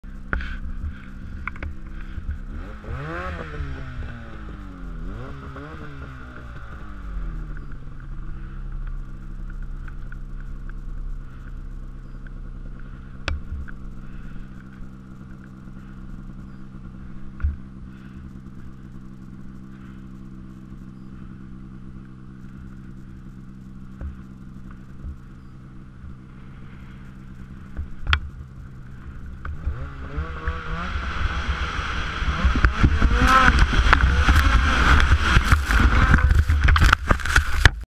Avalanche in the Snowies
Extreme avalanche ride in the snowies. Polaris Pro RMK 800 was ridden all the way down the avalanche.